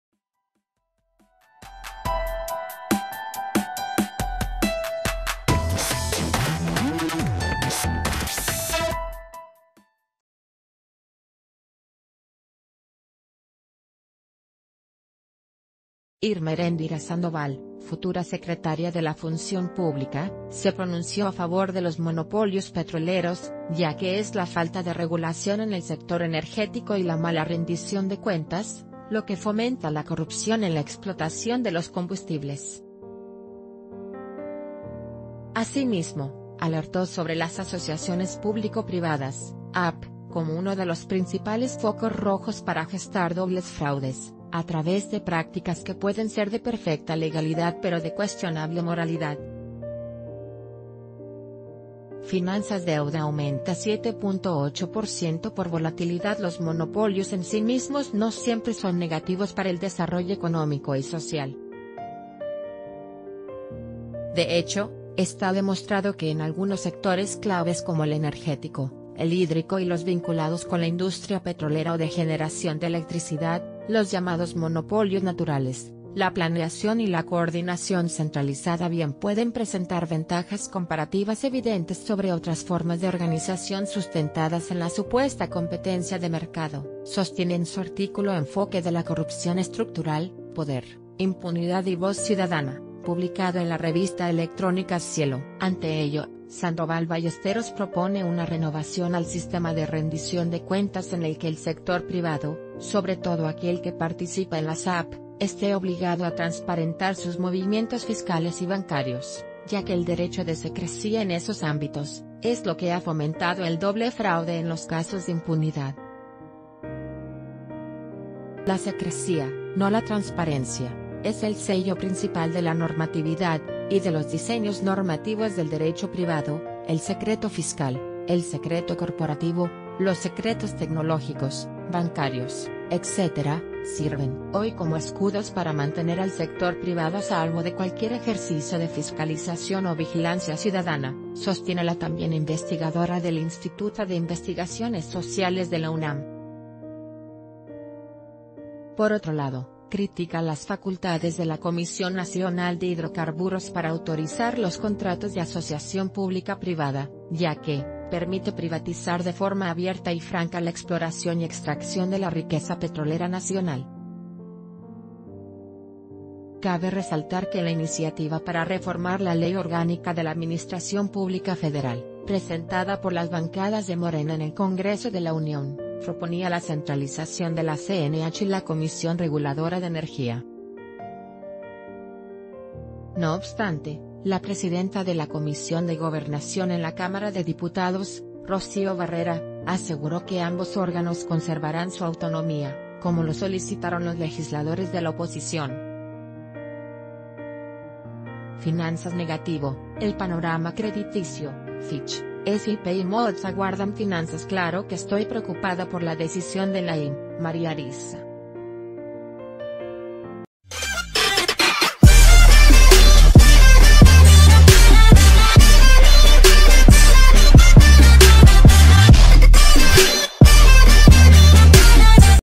Futura secretaria de la Función Pública, a favor de los monopolios petroleros
Irma Eréndira Sandoval, futura secretaria de la Función Pública, se pronunció a favor de los monopolios petroleros, ya que es la falta de regulación en el sector energético y la mala rendición de cuentas, lo que fomenta la corrupción en la explotación de los combustibles. Asimismo, alertó sobre las ...